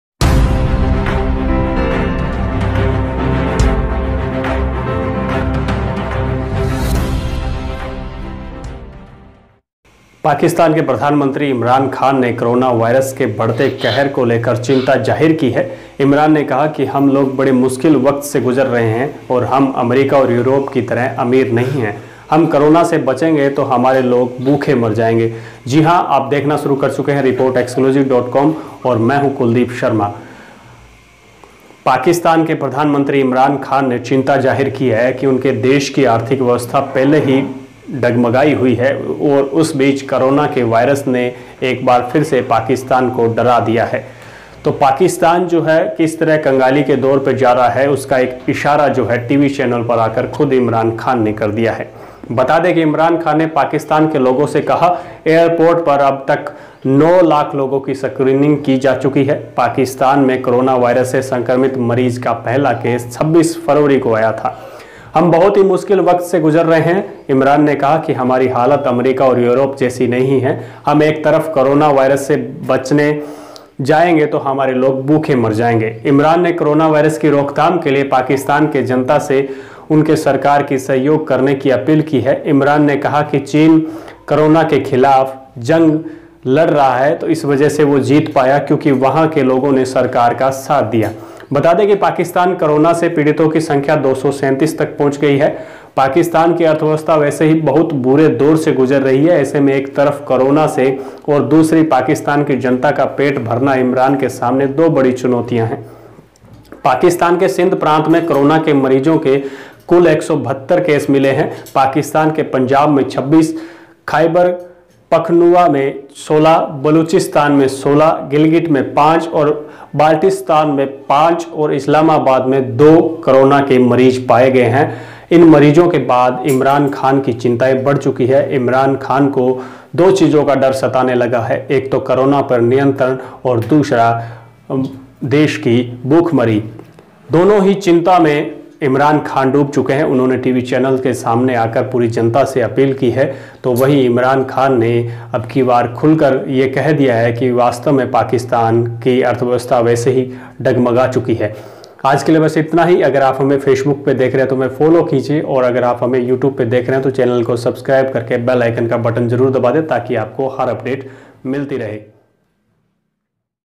The Prime Minister of Pakistan has finally accepted that his country is struggling with starvation
The Prime Minister of Pakistan has finally accepted that his country is struggling with starvation, Imran Khan appealed to the public